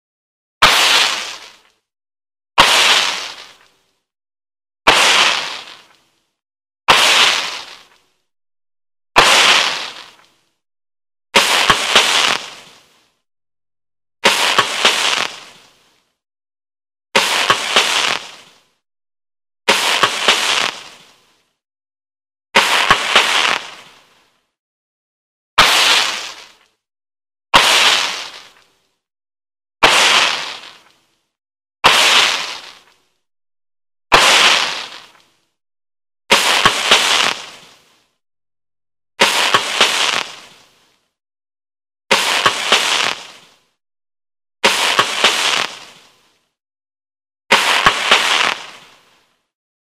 Искры электричества ⚡ - звук искр электричества и шум электроискр 🔌
Звук электроискр и шум искр электричества. 🔌 Узнай какие звуки издают искры электротока и как шумят искры электричества. ⚡